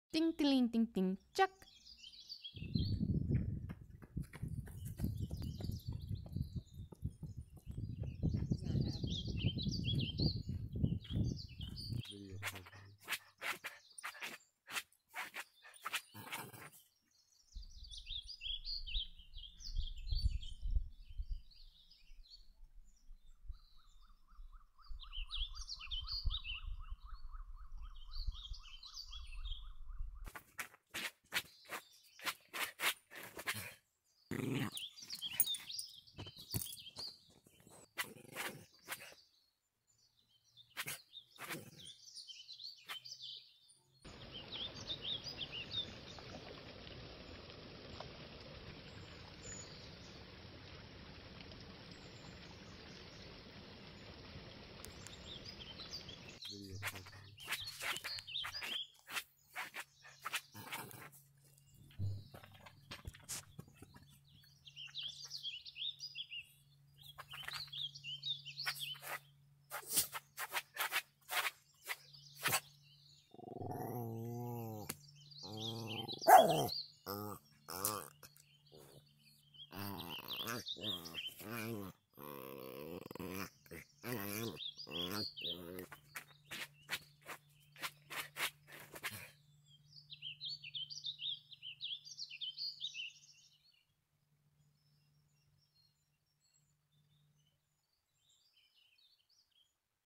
Pomeranian Fitness
Join this Pomeranian on his daily fitness routine in the LassenNationalForest.